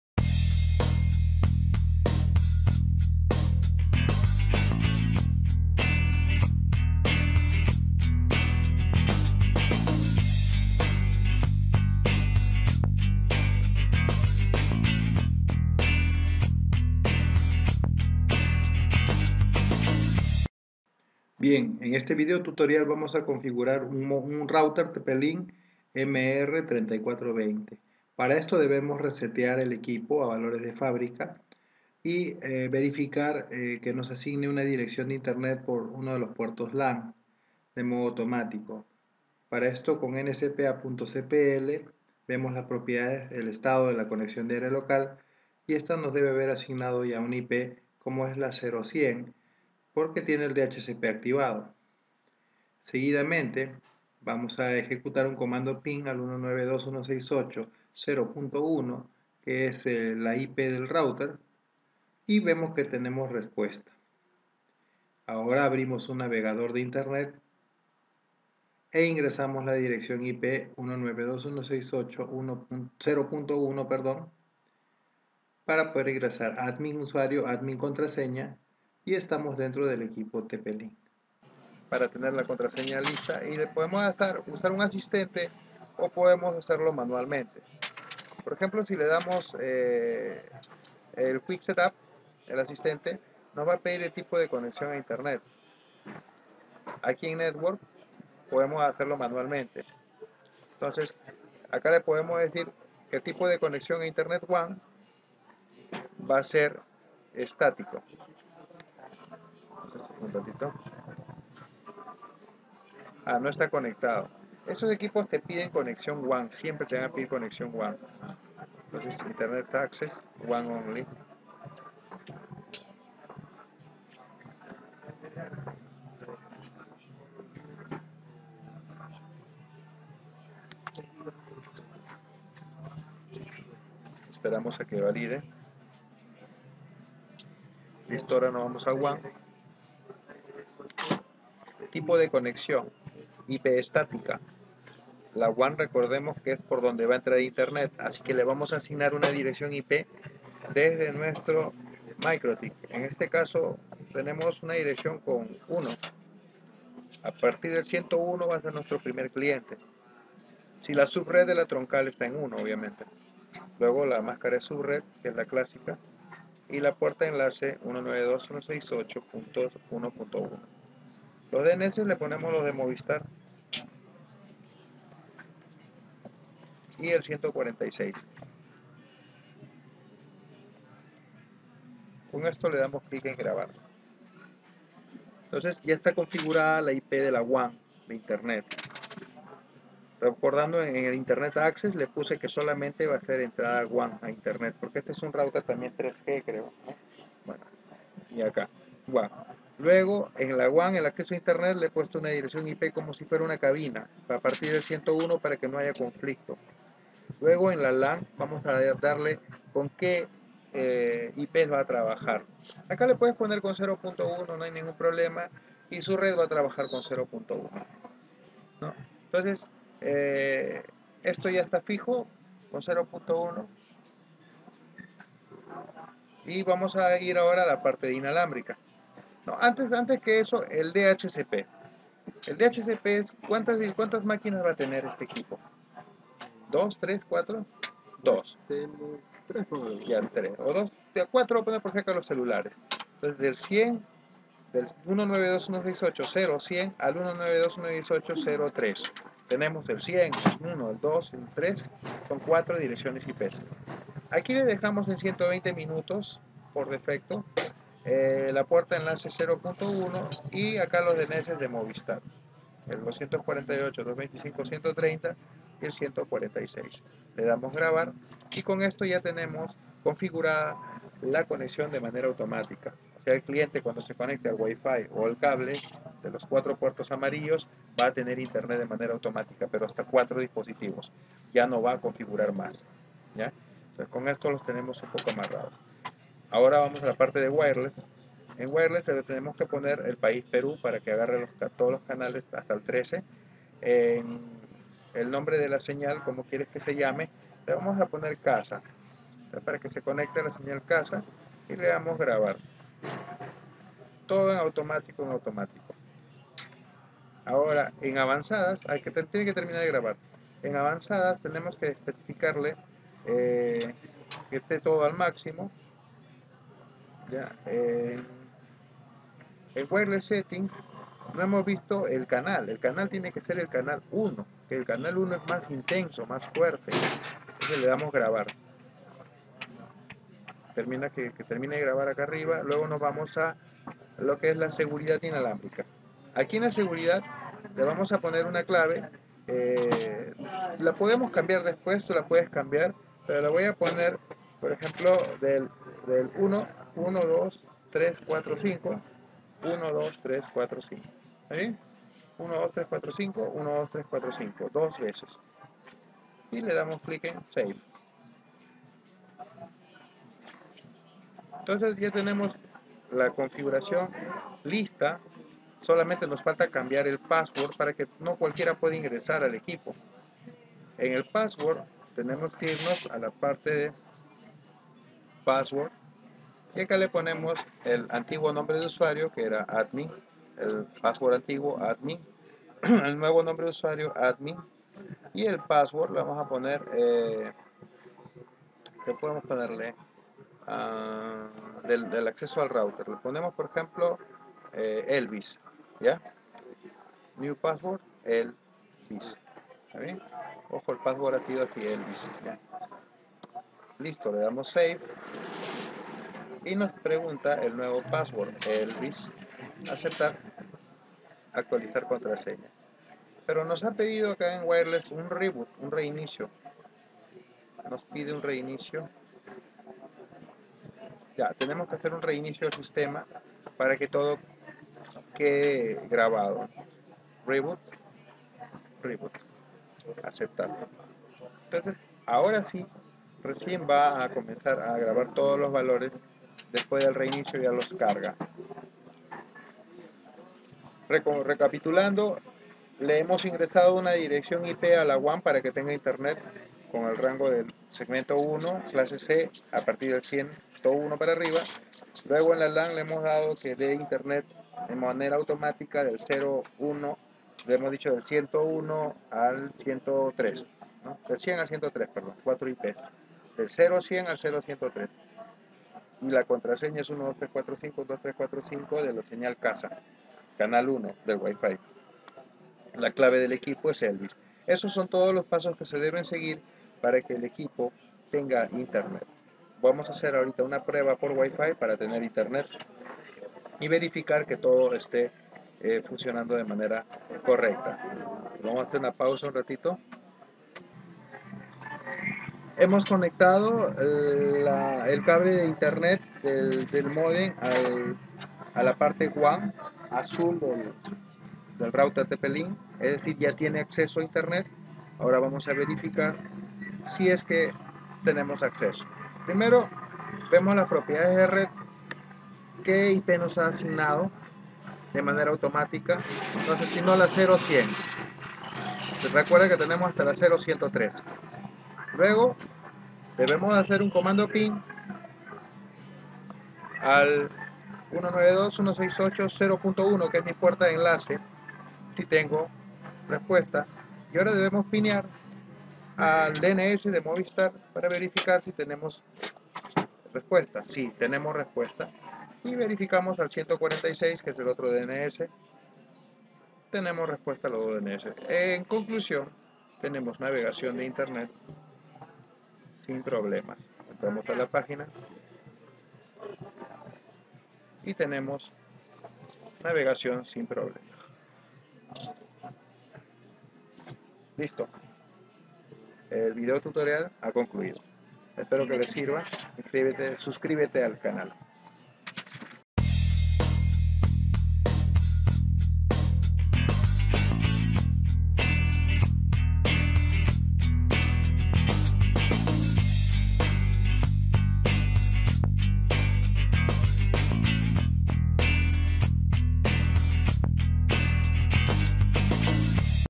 TPLINK MR3420 CONFIGURACIÓN RÁPIDA
En este video se configura la entrada wan para el acceso a internet  y las salidas lan para brindar ip automática a los usuarios de internet usando el TP LINK MR3420.